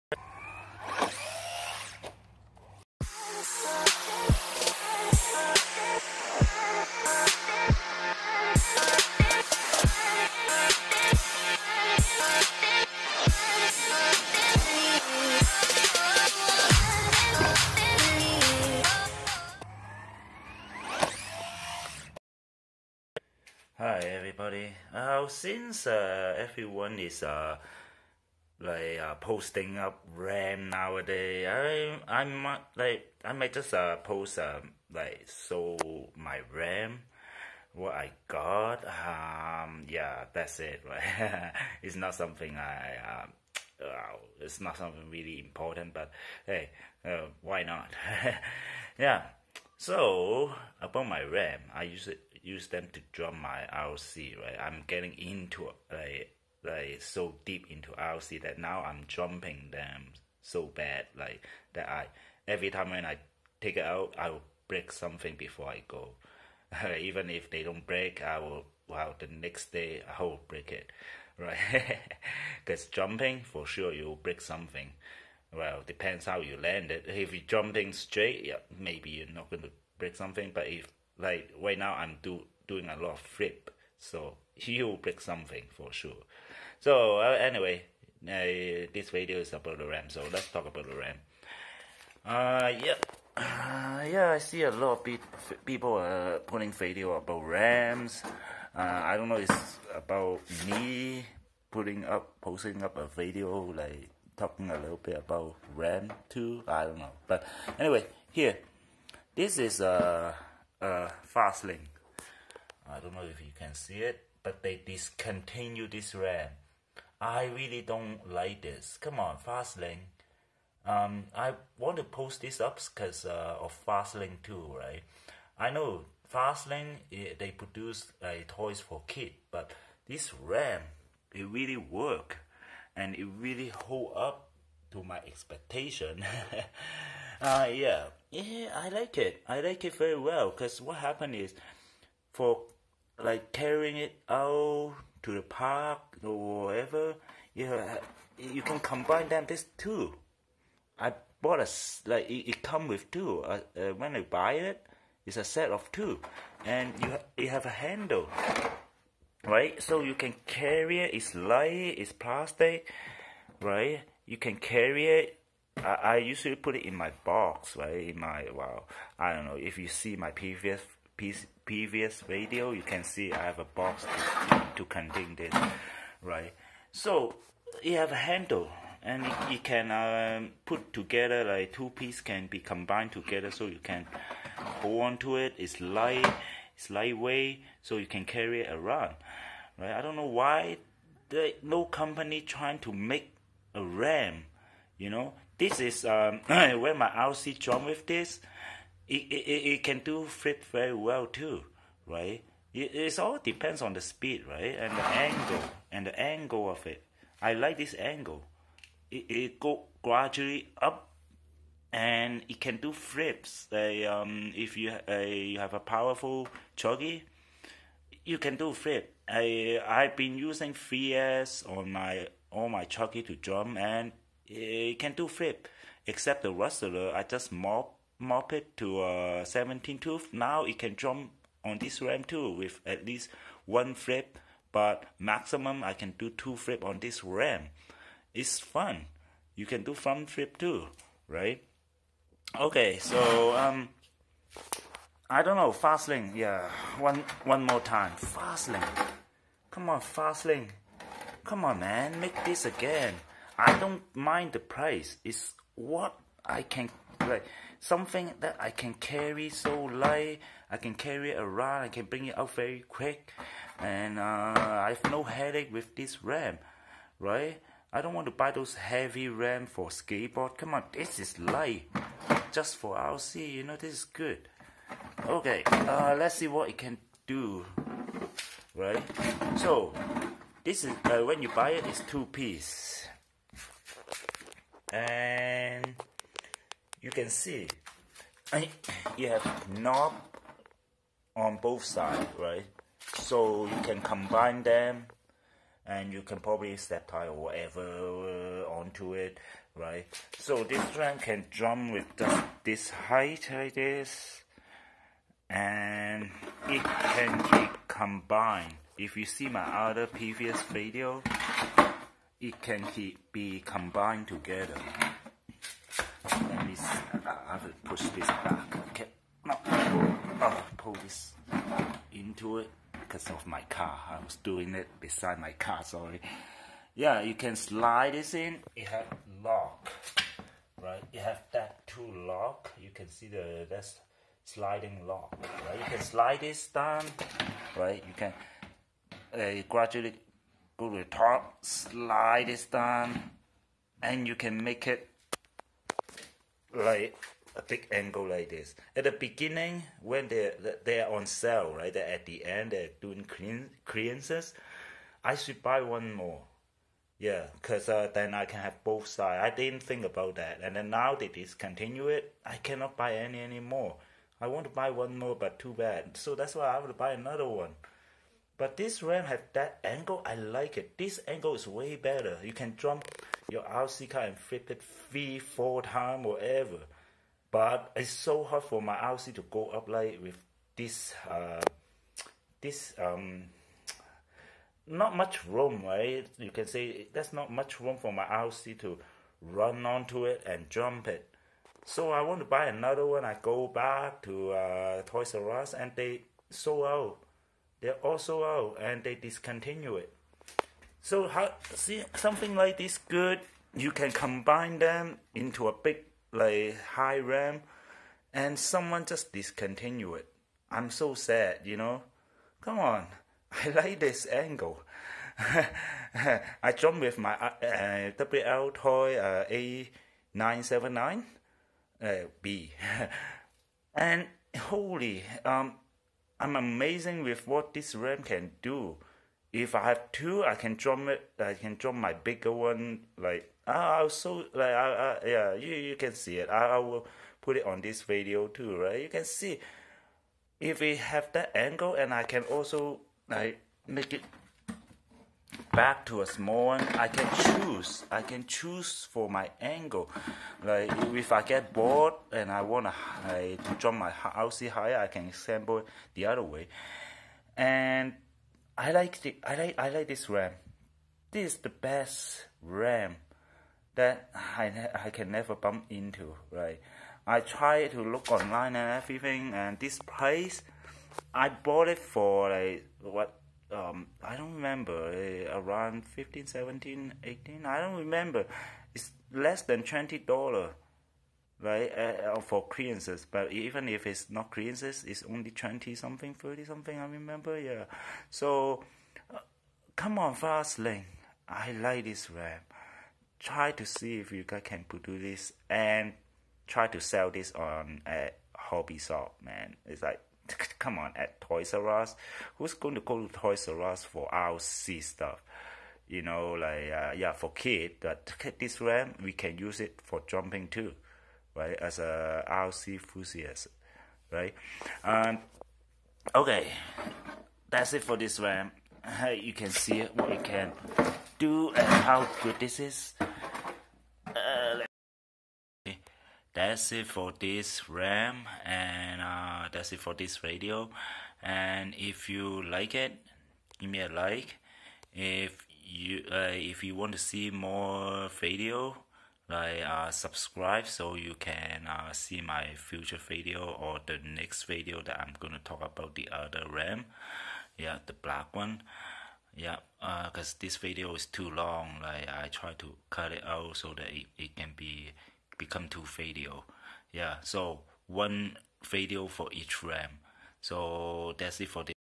Jumping Ramp for my Rc car (part 1) fast lane ramp
Fast lane or any plastic/metal company, produce a ramp like this with a fair price, also make a ramp that is easy to carry around and easy to put together.